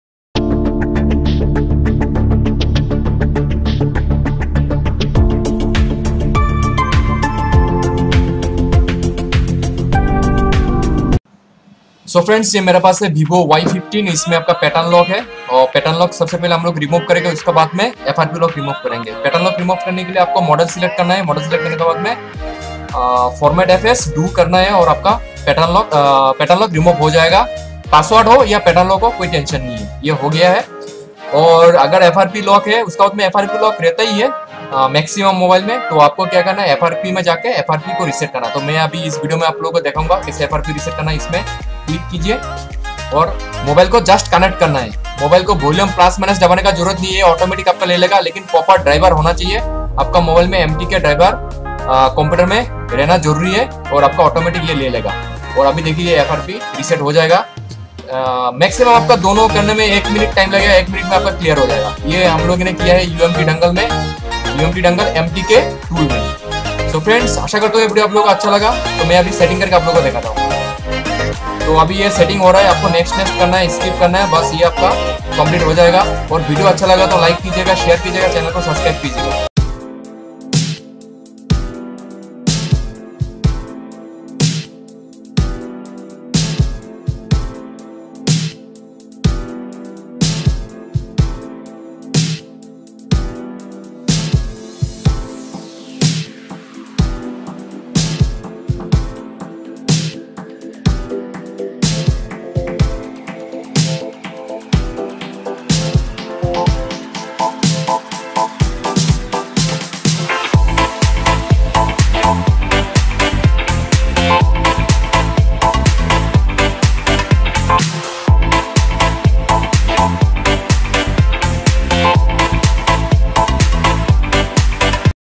VIVO Y15 PATTERN & FRP LOCK REMOVED IN JUST 1 MIN UMT DONGLE MTK TOOL
VIVO PASSWORD LOCK / PATTERN LOCK / FRP LOCK REMOVED UMT MULTI TOOL IS BEST TOOLS FOR MEDIATEK CPU.
YOUR PC SHOULD HAVE PROPER MTK DRIVER.
ANY MTK CPU TYPE MOBILE YOU CAN REMOVE LOCK EASILY
VIVO Y15 Y9101 Pattern Lock Reset by UMT No lose data 1000%
vivo y15 pattern unlock umt 100% Solution